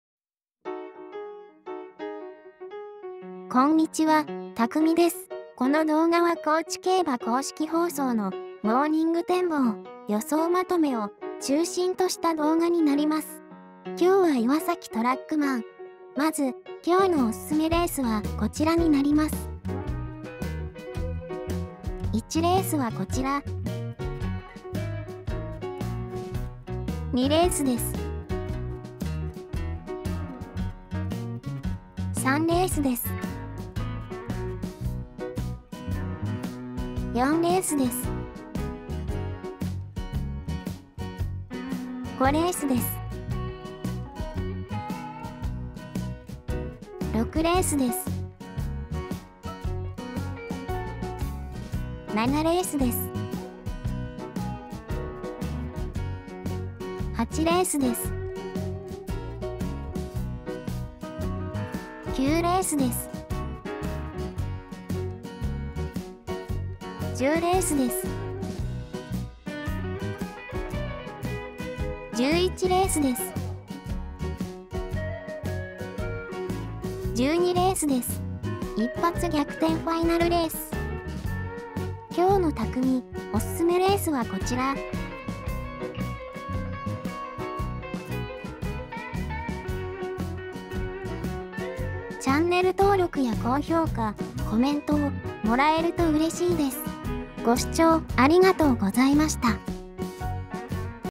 高知競馬予想まとめ2021.10.16（モー展。一発逆転ファイナルレース）
モー展。をメインとした予想まとめ動画です。
最後のたくみ予想はほとんどあたりません。
・高知競馬YouTubeチャンネル

高知競馬の詳細情報につきましては上記オフィシャルサイトや、
専門サイトをご覧ください。

キャラクター作成：カスタムキャスト
映像撮影：バーチャルキャスト
動画編集：Recotte Studio
音声：VOICEROID2 紲星あかり
HMD：Oculus Quest 2